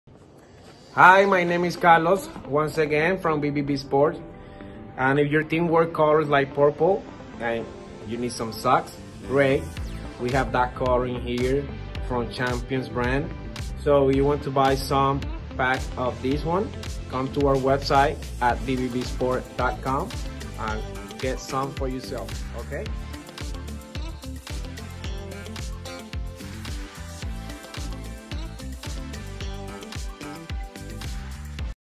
Champion Athletic Multi Sports Socks Purple (Pair) SK 🧦
Multi-Sport Socks Grab a pair of the Multi-Sport Socks, perfect for any sport.These socks are made from 100% Polyester and come in a standard tube style. Available in a variety of colors to match any uniform for any sport.